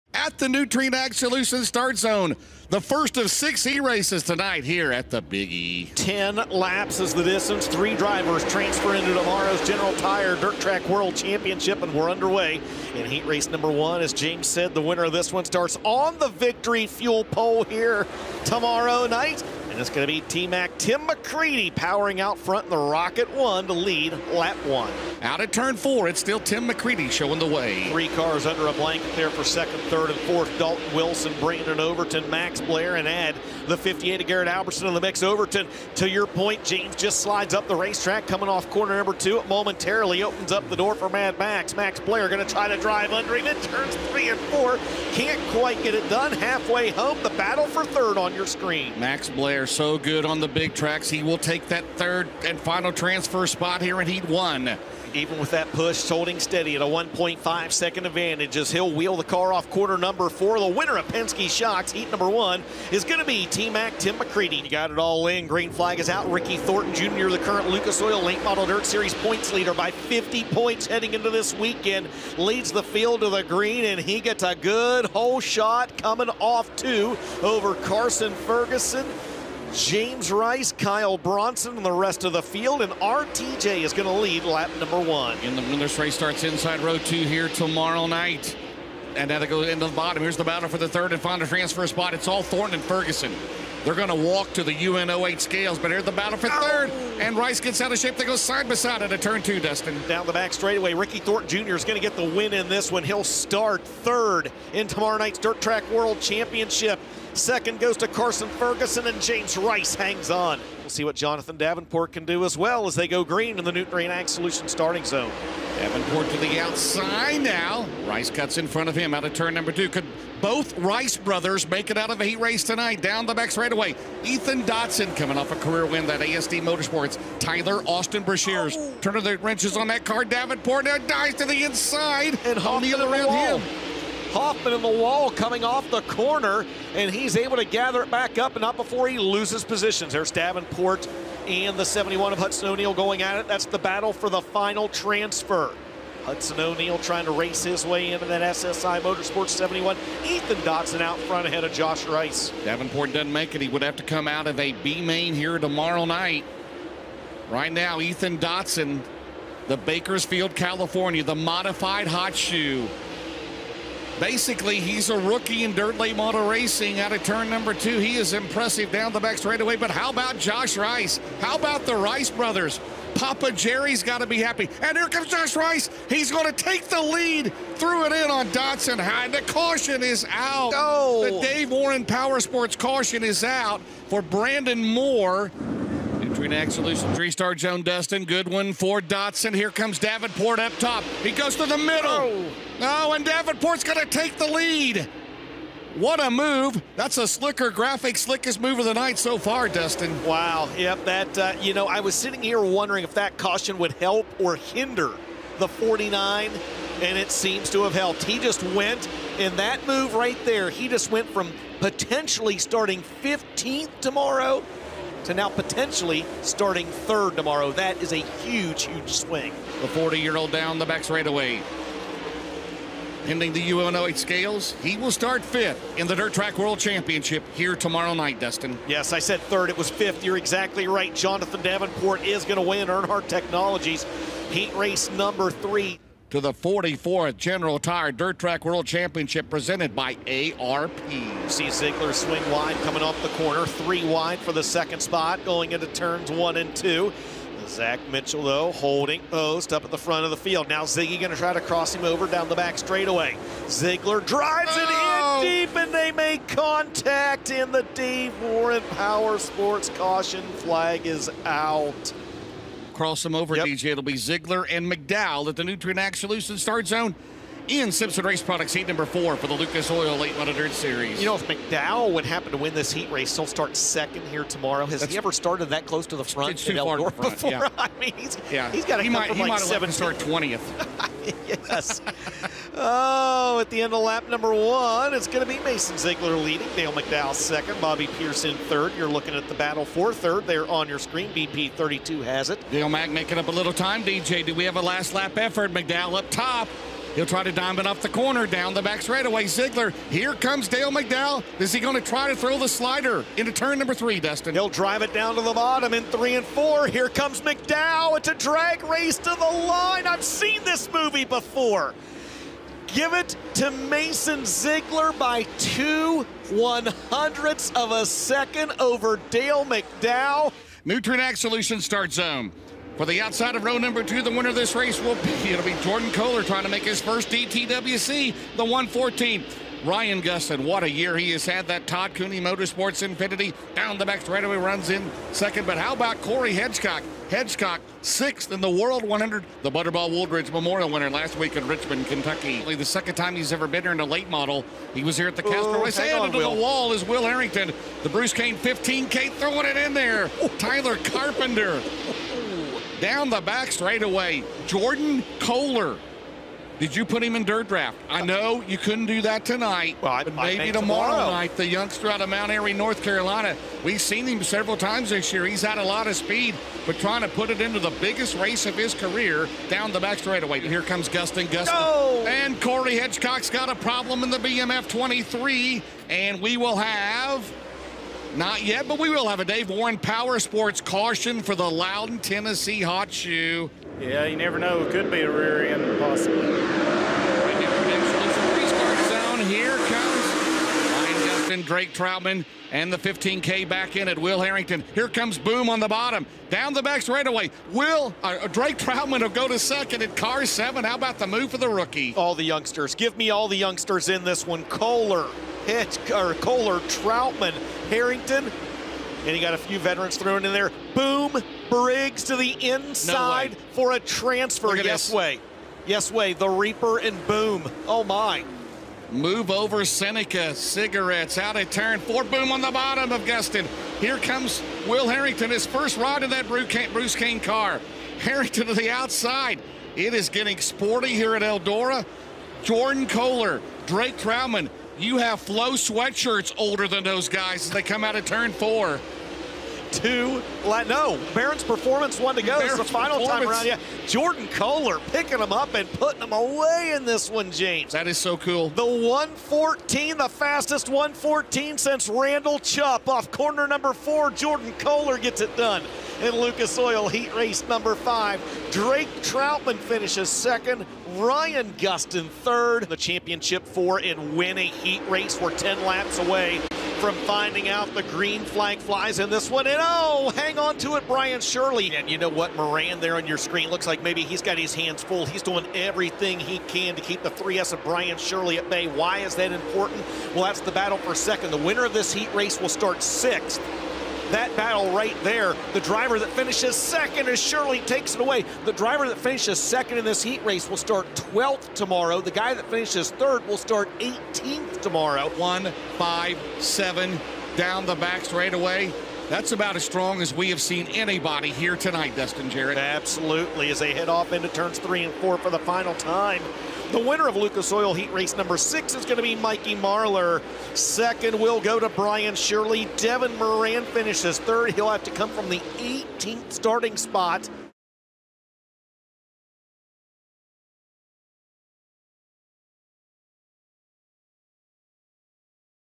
DTWC Heats | Lucas Oil Late Models at Eldora Speedway 10/18/24 | Highlights
The first 18 starting positions or the 44th Dirt Track World Championship are set via six Lucas Oil Late Model Dirt Series heat races at Eldora Speedway on Friday, October 18, 2024.